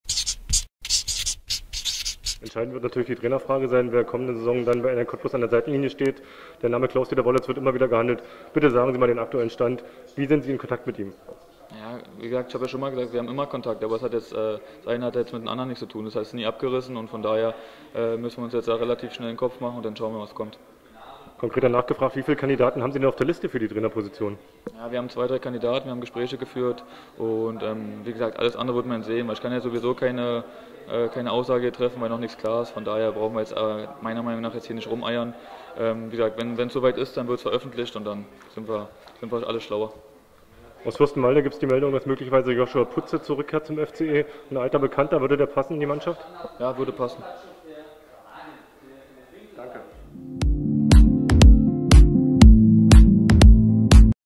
Kommen "Pele" Wollitz und Joshua Putze zu Energie Cottbus? Das sagt Sportchef Maximilian Zimmer
Kehren Trainer Claus-Dieter "Pele" Wollitz und Joshua Putze zu Energie Cottbus zurück? Das sagt Sportchef Maximilian Zimmer.